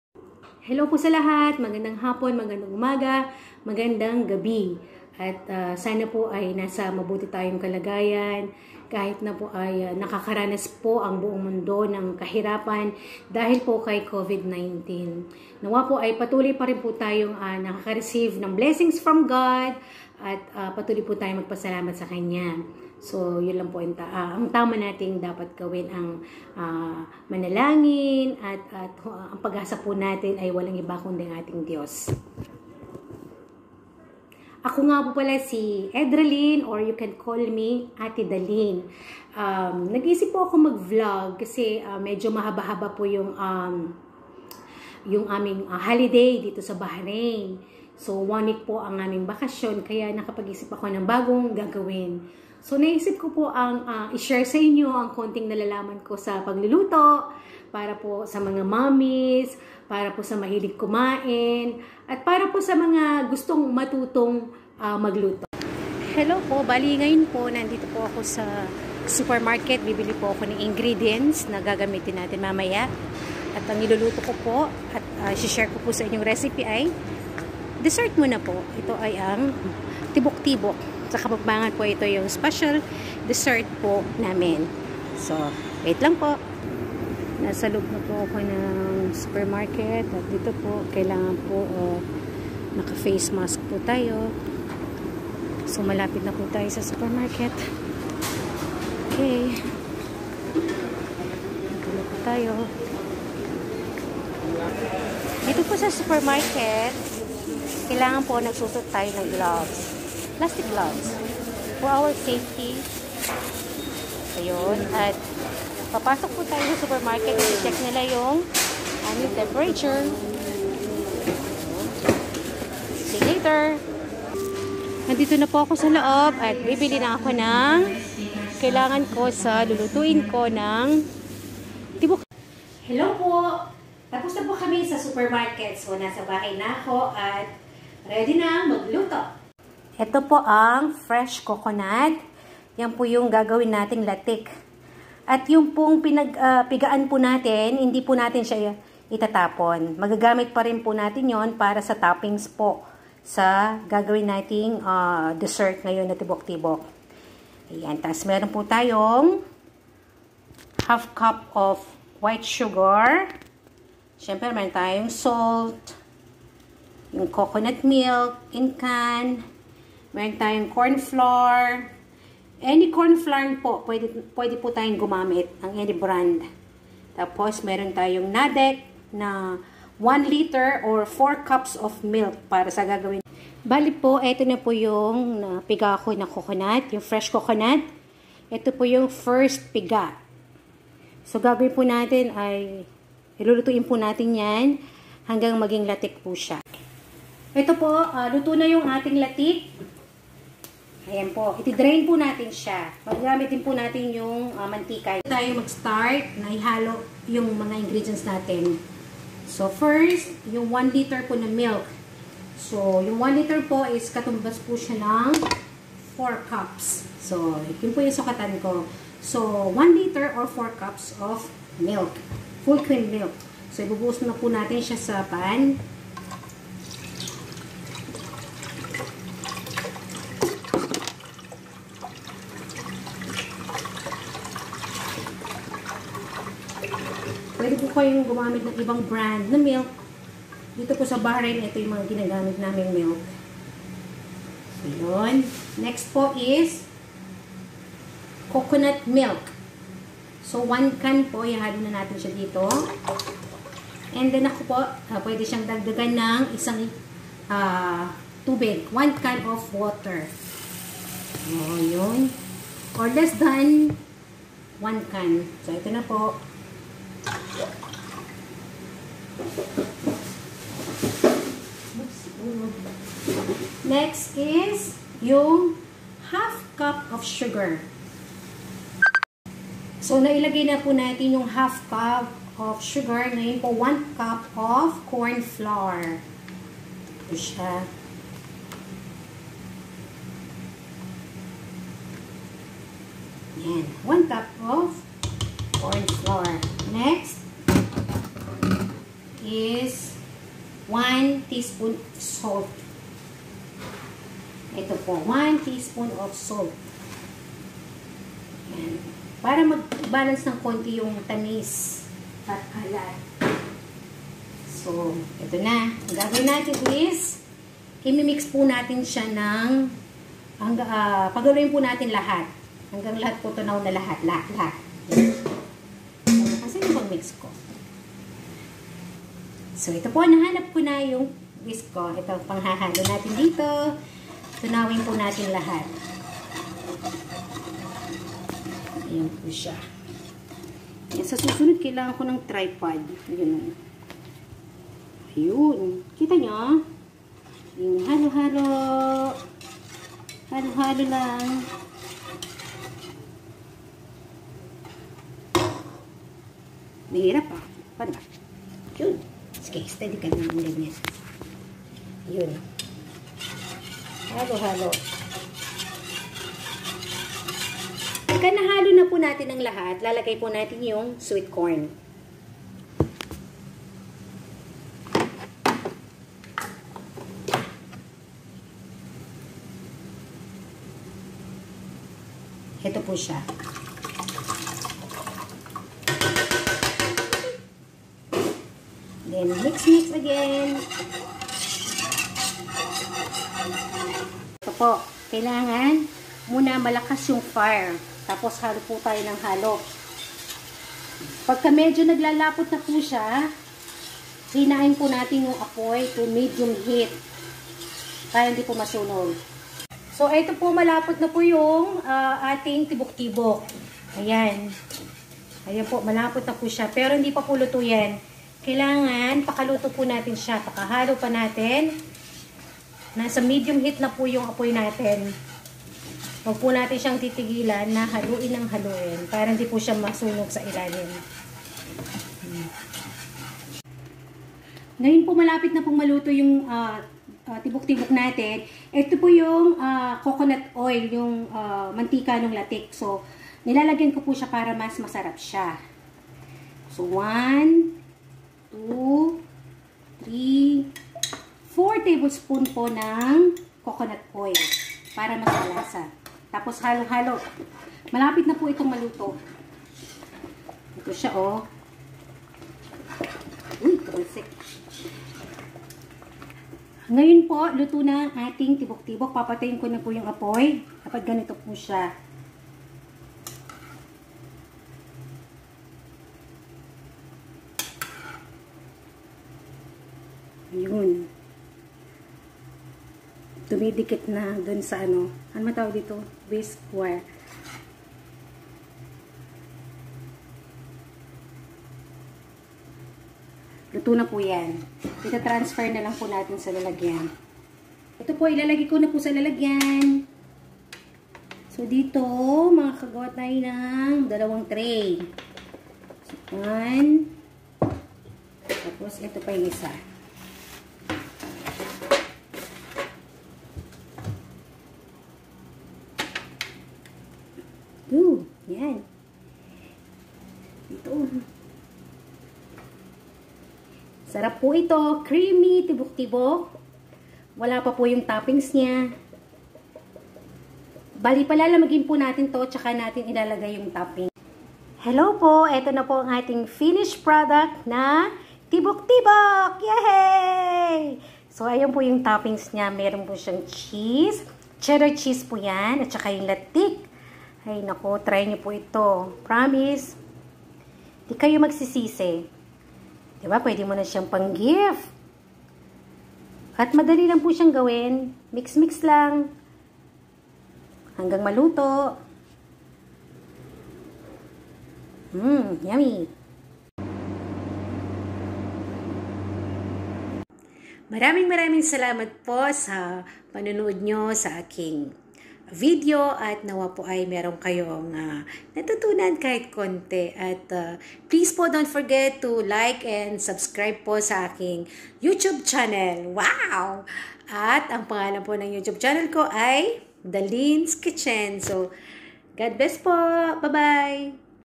Tibuk-Tibuk (Maja blanca) My first vlog😀❤ | Dalin's Kitchen
Dalin's Kitchen

Ingredients:
4 cups of full cream milk or 1 litre
1 can coconut milk
1/2 cup of water or evap milk
1/2 of sugar or according  to your taste
A pinch of salt
1 cup of cornflour
1 small can of creamy sweetcorn and whole kernel corn
4 tablespoons of coconut milk (optional)

Toppings:
Cheddar cheese
Latik (solid coconut curds)

Dalin's Kitchen